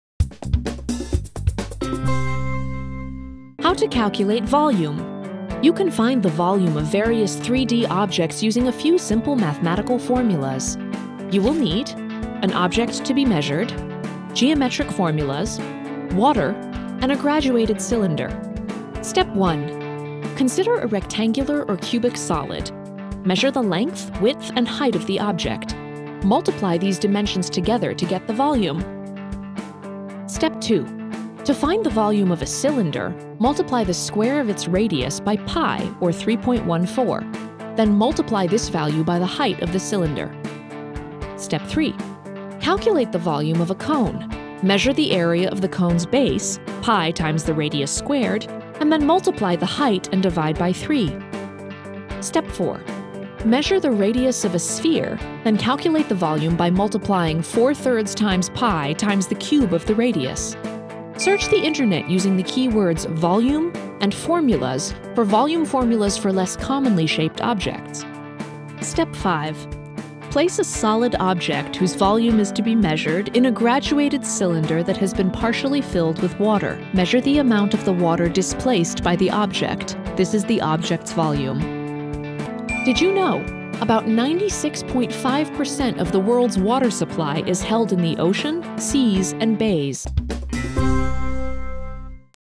How to Calculate Volume
You can find the volume of various 3-D objects using a few simple mathematical formulas.

Step 1: Consider a rectangular or cubic solid
Consider a rectangular or cubic solid. Measure the length, width, and height of the object. Multiply these dimensions together to get the volume.

Step 2: Look at a cylindrical object
To find the volume of a cylinder, multiply the square of its radius by pi, or 3.14. Then multiply this value by the height of the cylinder.

Step 3: Calculate the volume of a cone
Calculate the volume of a cone. Measure the area of the cone's base -- pi times the radius squared -- then multiply by the height and divide by 3.

Step 4: Measure a sphere
Measure the radius of a sphere, then calculate the volume by multiplying 4/3 times pi times the cube of the radius.

Tip
Search the internet using the key words "volume" and "formulas" for volume formulas for less commonly shaped objects.

Step 5: Use water displacement
Place a solid object whose volume is to be measured in a graduated cylinder that has been partially filled with water. Measure the amount of water displaced by the object -- this is the object's volume.

About 96.5% of the world's water supply is held in the ocean, seas, and bays.